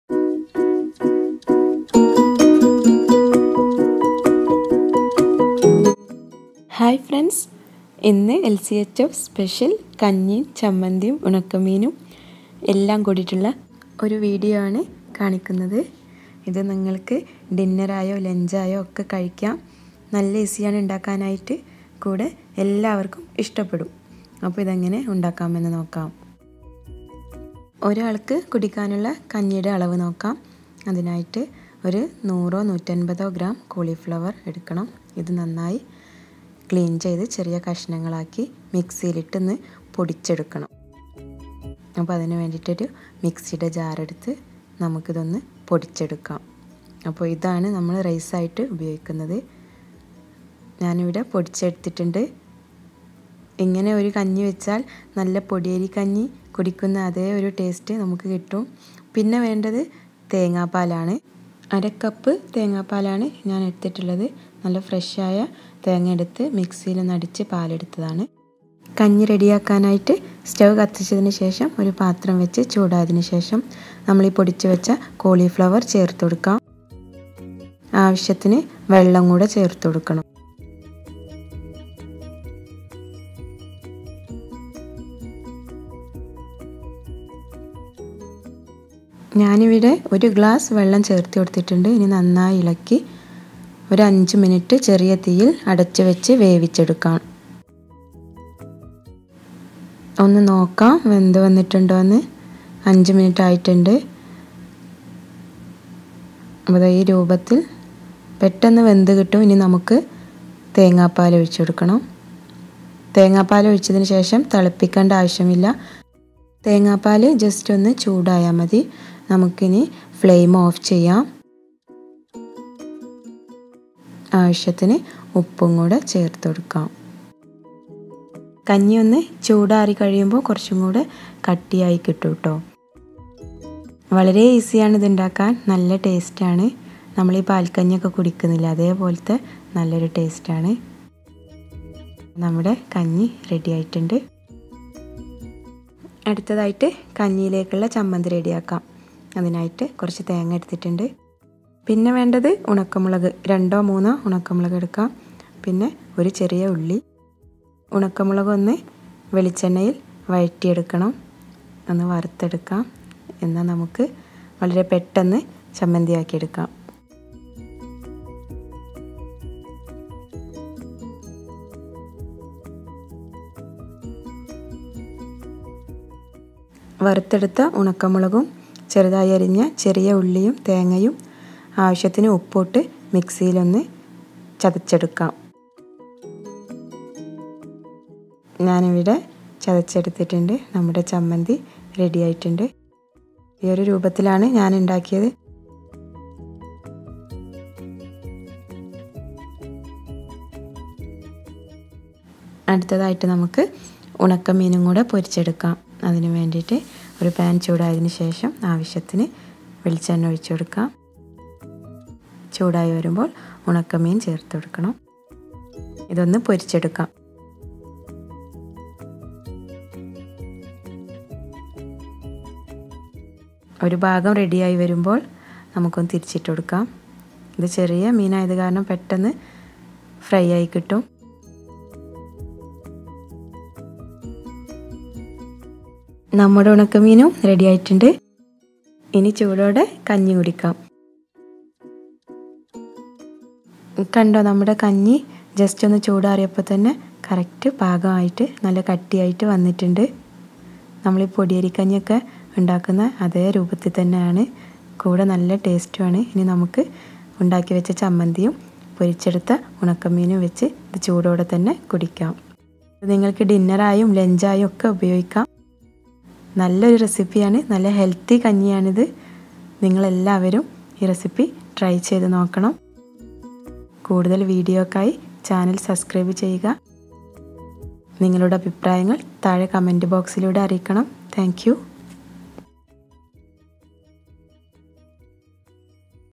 LCHF സ്പെഷ്യൽ കഞ്ഞീം ഉണക്ക മീനും ചമ്മന്തിയും||The Complete Lunch &Dinner||LCHF/KETO Recipe Malayalam
lchf ഡയറ്റ് ചെയ്യുന്നവർക്ക് അരികൊണ്ടുള്ള കഞ്ഞിക്ക് പകരമായിട്ട് കാളിഫ്ലവർ വെച് ഉണ്ടാക്കുന്ന നല്ല പൊടിയരി പാൽ കഞ്ഞി 100% ഹെൽത്തി
ഇത് ഉച്ചക്കും രാത്രിയിലും നമുക്ക് കഴിക്കാം